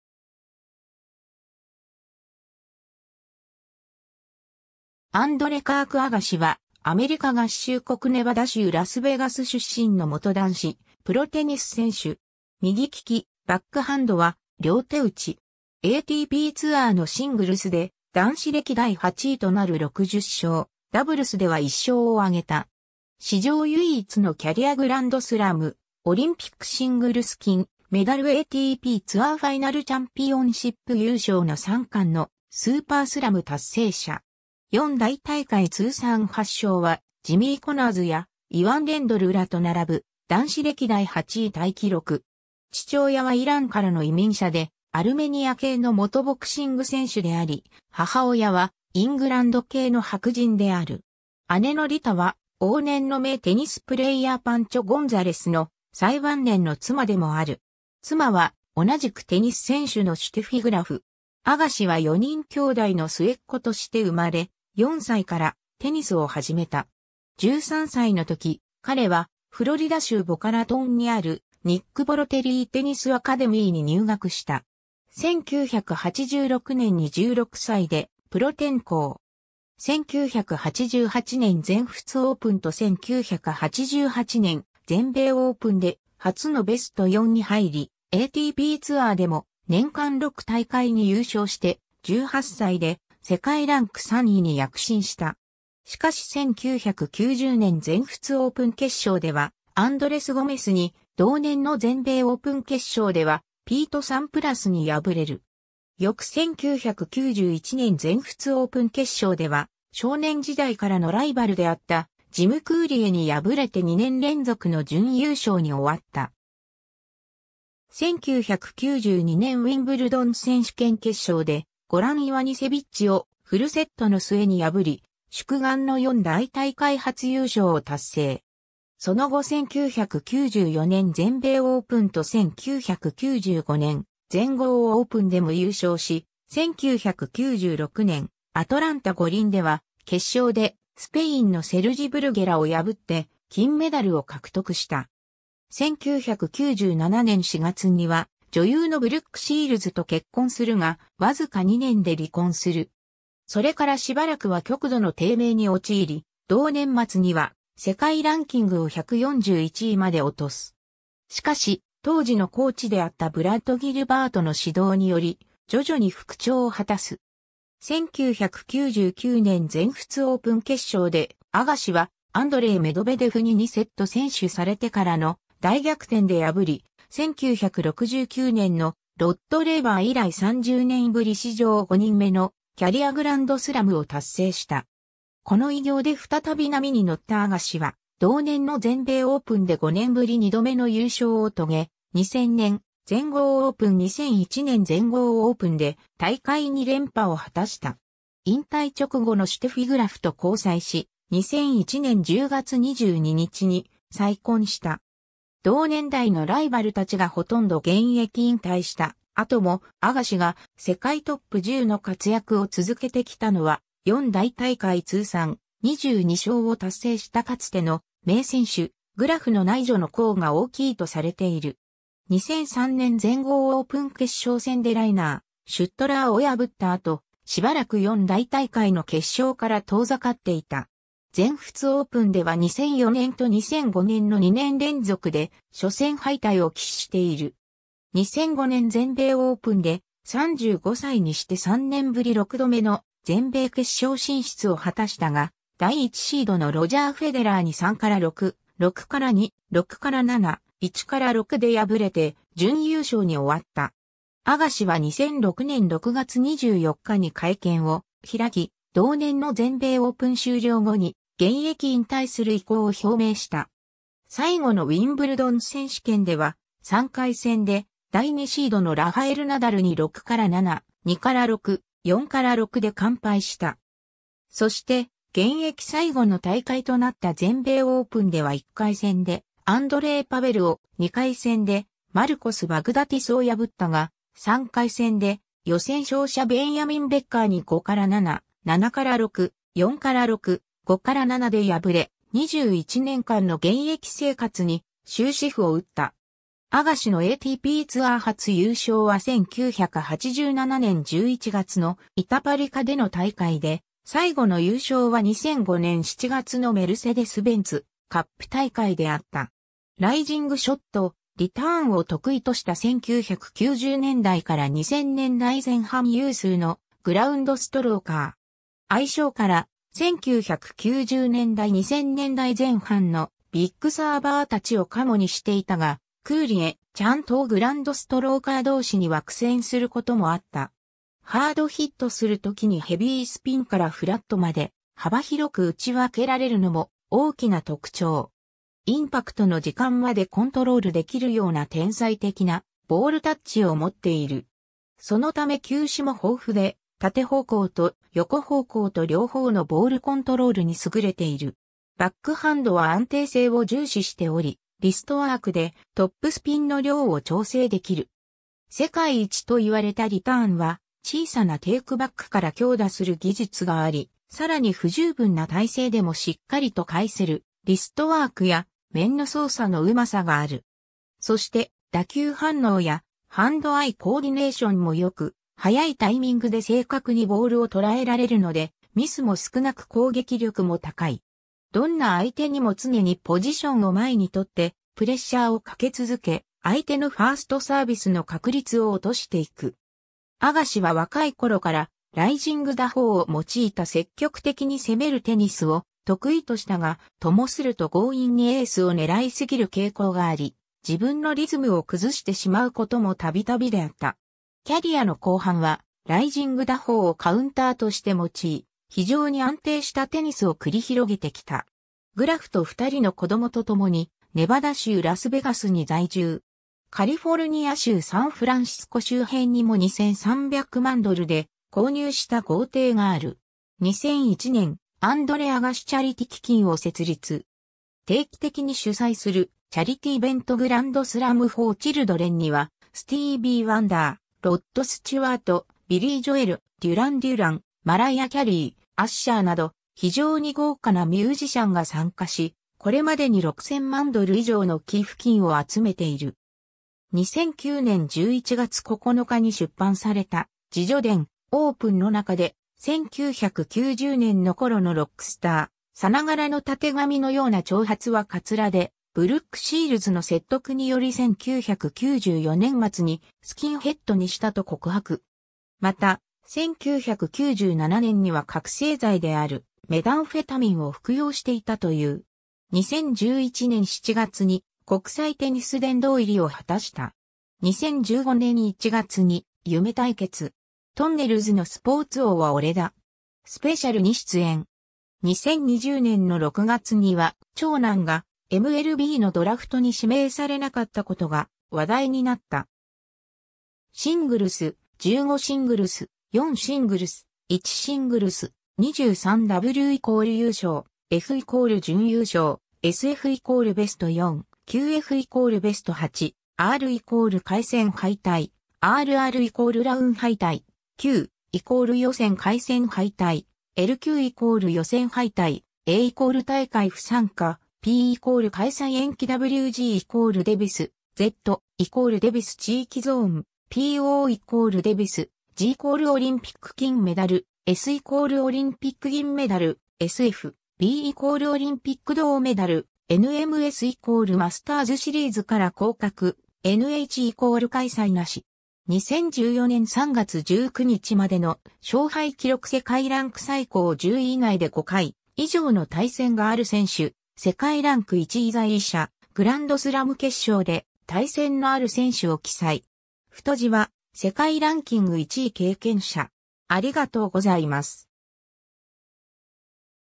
アンドレ・アガシ
アンドレ・カーク・アガシ（Andre Kirk Agassi, 1970年4月29日 - ）は、アメリカ合衆国ネバダ州ラスベガス出身の元男子プロテニス選手。
右利き、バックハンドは両手打ち。
ATPツアーのシングルスで男子歴代8位となる60勝、ダブルスでは1勝を挙げた。
 史上唯一のキャリア・グランドスラム・オリンピックシングルス金メダル・ATPツアーファイナルチャンピオンシップ優勝の3冠のスーパースラム達成者。
4大大会通算8勝は、ジミー・コナーズやイワン・レンドルらと並ぶ男子歴代8位タイ記録 。
 父親はイランからの移民者で、アルメニア系の元ボクシング選手であり、母親はイングランド系の白人である。
姉のリタは往年の名テニスプレーヤーパンチョ・ゴンザレスの最晩年の妻でもある。
 妻は、同じくテニス選手のシュテフィ・グラフ。
 アガシは4人兄弟の末っ子として生まれ、4歳からテニスを始めた。
13歳の時、彼はフロリダ州ボカラトンにあるニック・ボロテリー・テニスアカデミーに入学した。
1986年に16歳でプロ転向。
1988年全仏オープンと1988年全米オープンで初のベスト4に入り、ATPツアーでも年間6大会に優勝して、18歳で世界ランク3位に躍進した。
しかし1990年全仏オープン決勝ではアンドレス・ゴメスに、同年の全米オープン決勝ではピート・サンプラスに敗れる。
翌1991年全仏オープン決勝では、少年時代からのライバルであったジム・クーリエに敗れて2年連続の準優勝に終わった。
 1992年ウィンブルドン選手権決勝でゴラン・イワニセビッチをフルセットの末に破り、宿願の4大大会初優勝を達成。
その後1994年全米オープンと1995年全豪オープンでも優勝し、1996年アトランタ五輪では決勝でスペインのセルジ・ブルゲラを破って金メダルを獲得した。
 1997年4月には女優のブルック・シールズと結婚するが、わずか2年で離婚する。
 それからしばらくは極度の低迷に陥り、同年末には世界ランキングを141位まで落とす。
しかし、当時のコーチであったブラッド・ギルバートの指導により、徐々に復調を果たす。
1999年全仏オープン決勝で、アガシはアンドレイ・メドベデフに2セット先取されてからの大逆転で破り、1969年のロッド・レーバー以来30年ぶり史上5人目のキャリア・グランドスラムを達成した。
この偉業で再び波に乗ったアガシは、同年の全米オープンで5年ぶり2度目の優勝を遂げ、2000年全豪オープン・2001年全豪オープンで大会2連覇を果たした。
 引退直後のシュテフィ・グラフと交際し、2001年10月22日に再婚した。
同年代のライバルたちがほとんど現役引退した後も、アガシが世界トップ10の活躍を続けてきたのは、4大大会通算「22勝」を達成したかつての名選手、グラフの内助の功が大きいとされている。
 2003年全豪オープン決勝戦でライナー・シュットラーを破った後、しばらく4大大会の決勝から遠ざかっていた。
全仏オープンでは2004年と2005年の2年連続で初戦敗退を喫している。
2005年全米オープンで、35歳にして3年ぶり6度目の全米決勝進出を果たしたが、第1シードのロジャー・フェデラーに3-6, 6-2, 6-7, 1-6で敗れて準優勝に終わった。
 アガシは2006年6月24日に会見を開き、同年の全米オープン終了後に現役引退する意向を表明した。
最後のウィンブルドン選手権では、3回戦で第2シードのラファエル・ナダルに6-7, 2-6, 4-6で完敗した。
そして、現役最後の大会となった全米オープンでは1回戦でアンドレイ・パベルを、2回戦でマルコス・バグダティスを破ったが、3回戦で予選勝者ベンヤミン・ベッカーに5-7, 7-6, 4-6, 5-7で敗れ、21年間の現役生活に終止符を打った。
 アガシのATPツアー初優勝は1987年11月のイタパリカでの大会で、最後の優勝は2005年7月のメルセデスベンツ・カップ大会であった。
 ライジングショット、リターンを得意とした1990年代から2000年代前半有数のグラウンドストローカー。
 相性から、1990年代...